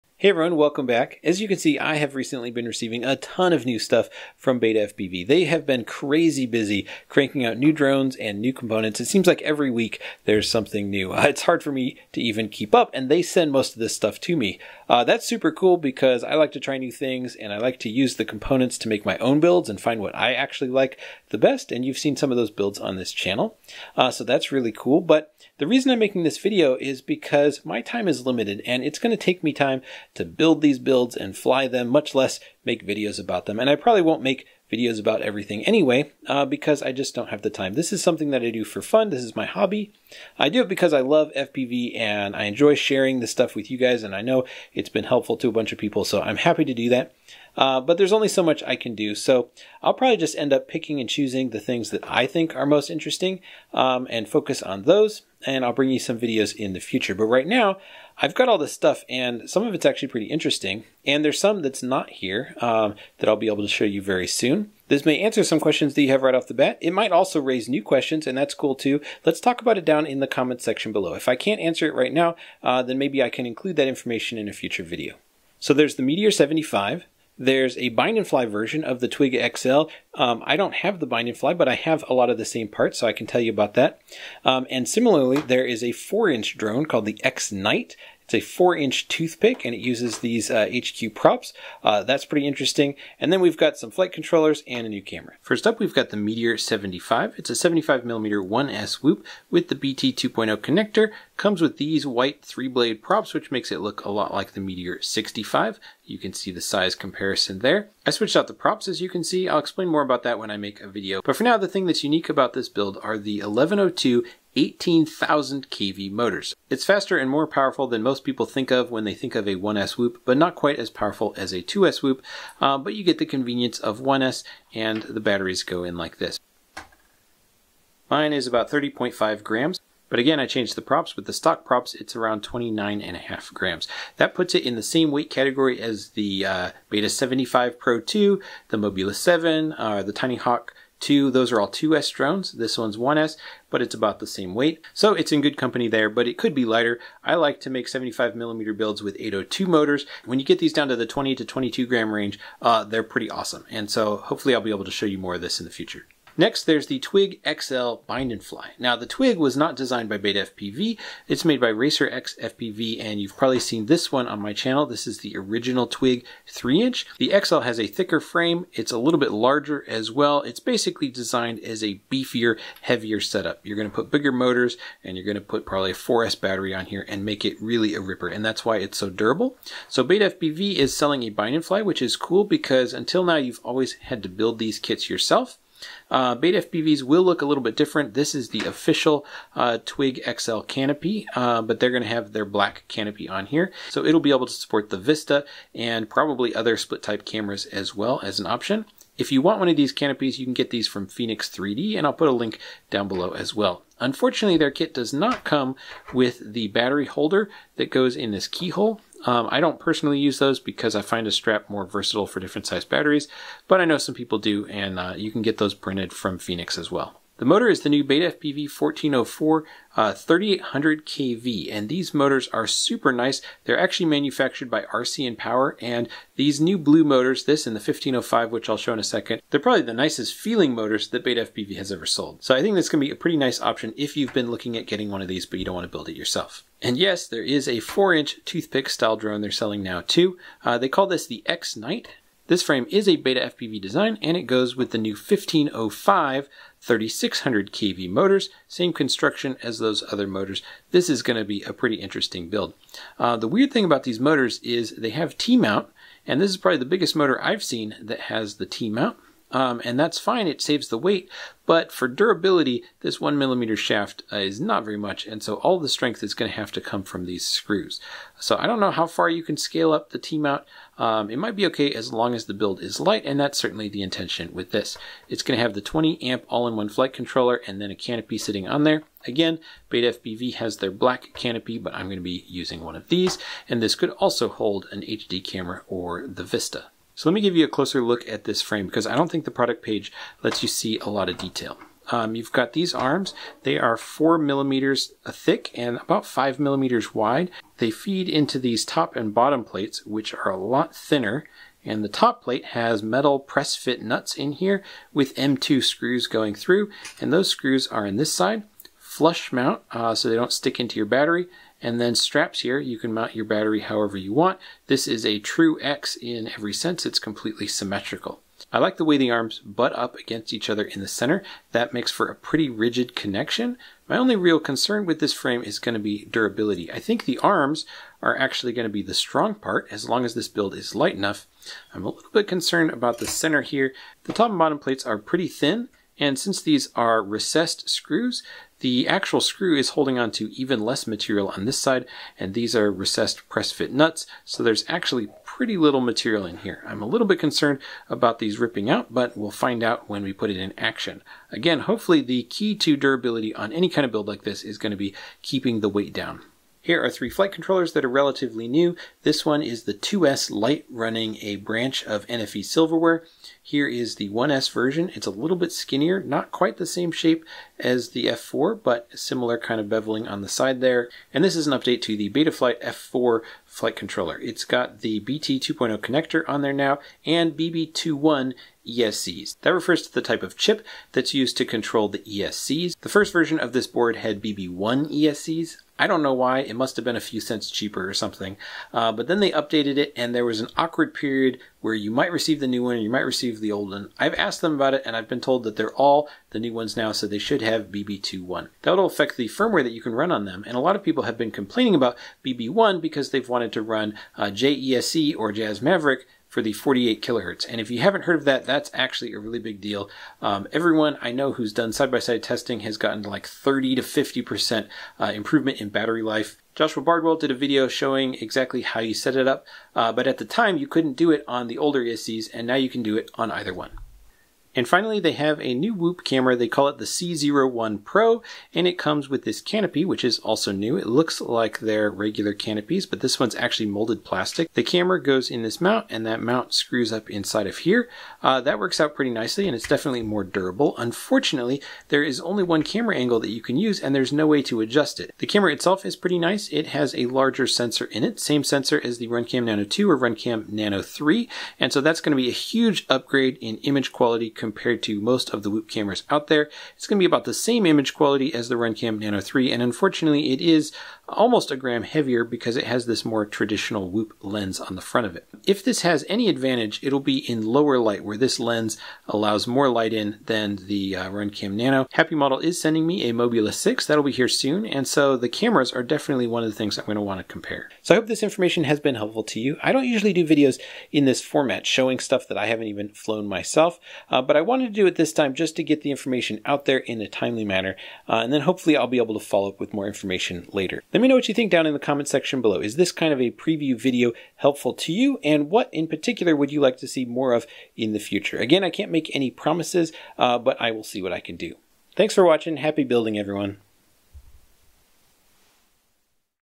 4" Toothpick, Meteor75, and More Stuff Coming!
BETAFPV has been crazy busy making new drones and new components. I have some of them here, so I can give you a quick preview. More info to follow after I get more hands on experience. Links below for full product details...